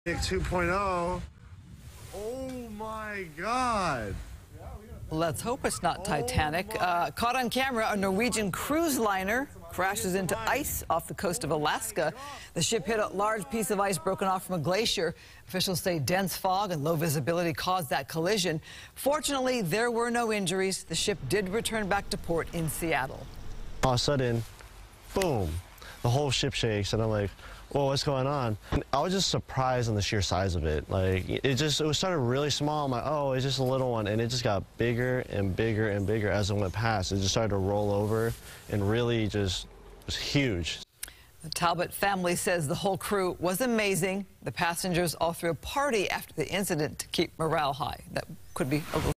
Cruise ship hits iceberg near Alaska
Footage shows a Norwegian cruise ship hitting an iceberg near Hubbard Glacier in the waters off southern Alaska.